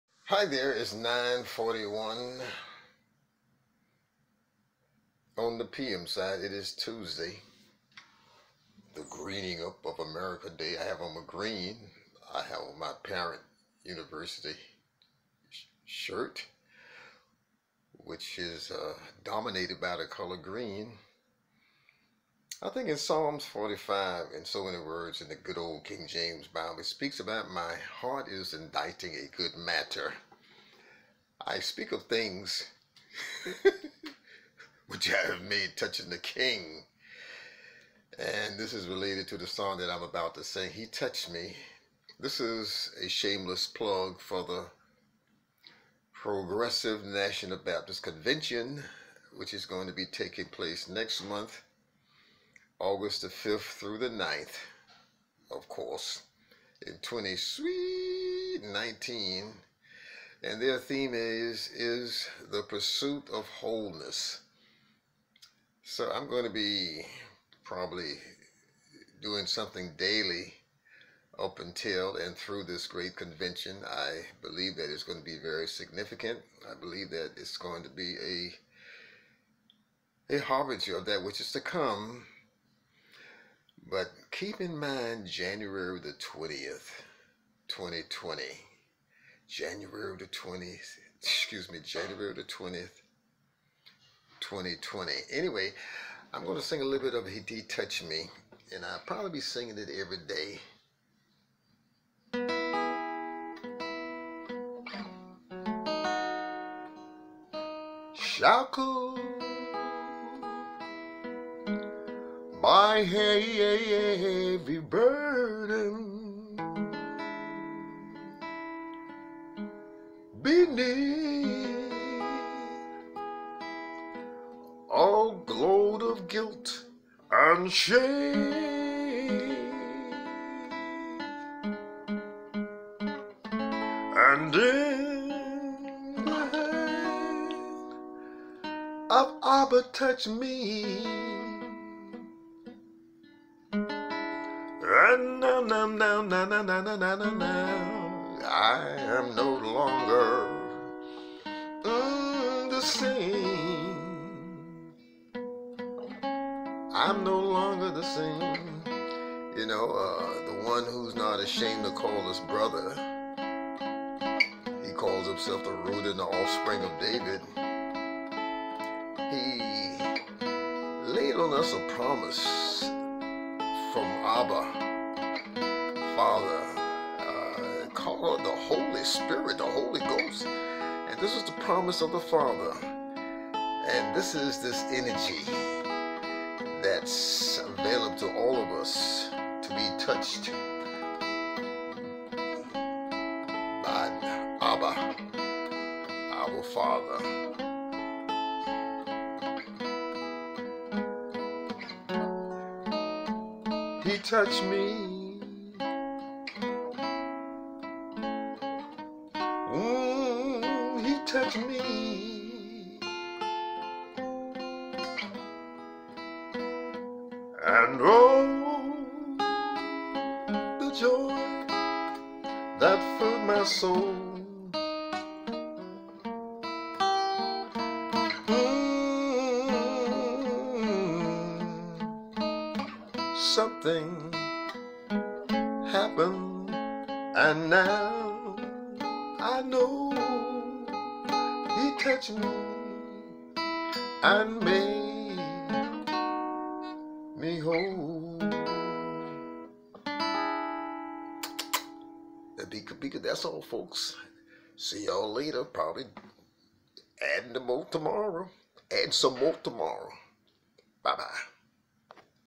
DEEPY'S ANTICIPATORY SUNNY FAITH WHEN IT COMES TO THE PROGRESSIVE NATIONAL BAPTIST CONVENTION
Spring Sunnyfully Anew!

Be there all, here, now in person and/or in consciousness.

The Gold: THE PROGRESSIVE NATIONAL
                  BAPTIST CONVENTION
Theme: "IN PURSUIT OF WHOLENESS"
Dates August 5-9, 2019
Place: Lady Sweet Atlanta, G-a